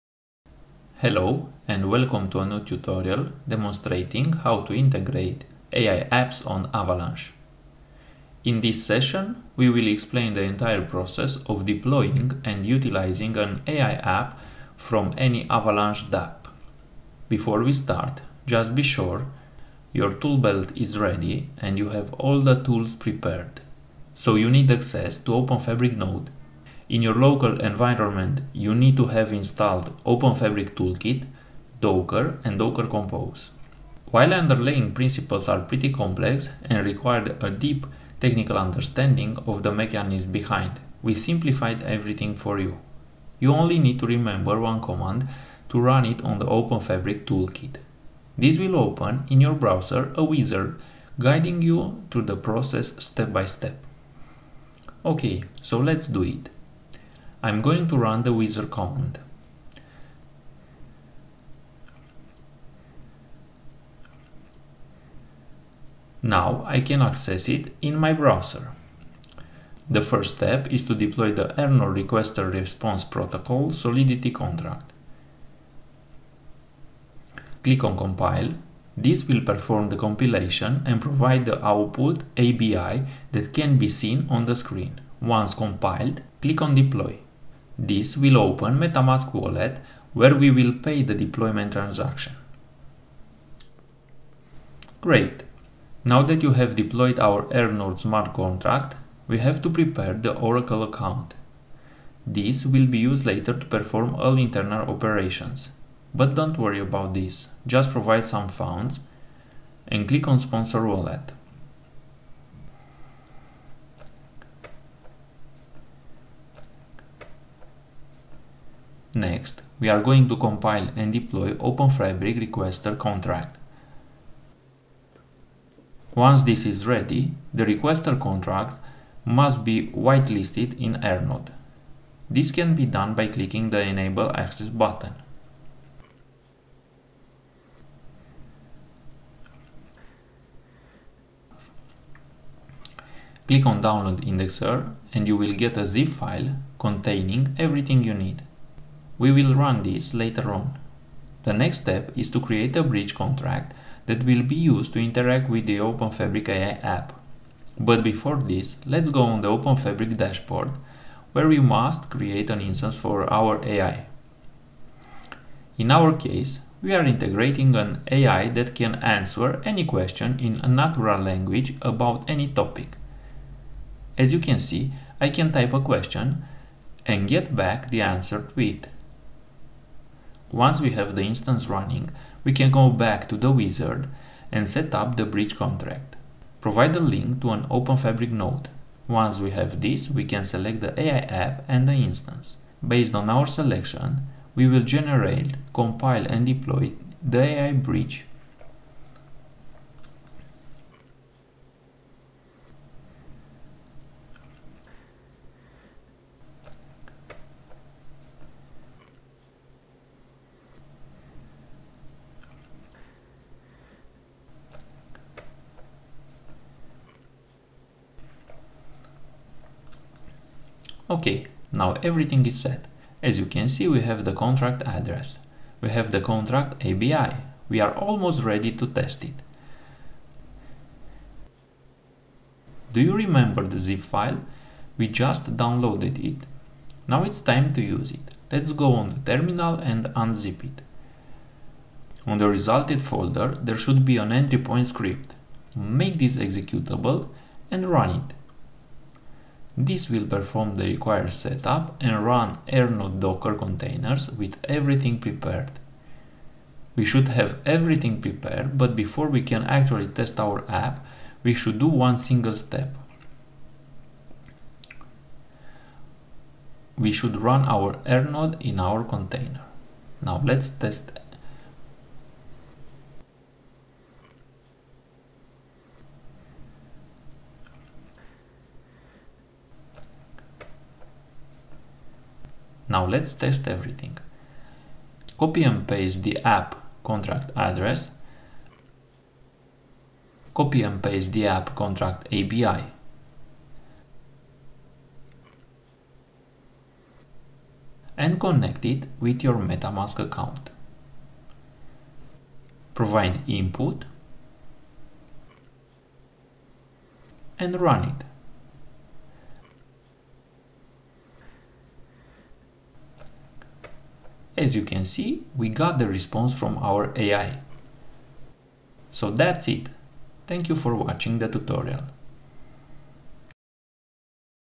Integrate AI-App on Avalanche
Openfabric, a new ecosystem for building and connecting AI Apps, is now seamlessly connected to Avalanche, an open, programmable smart contracts platform for decentralized applications.

The Internet will never be the same.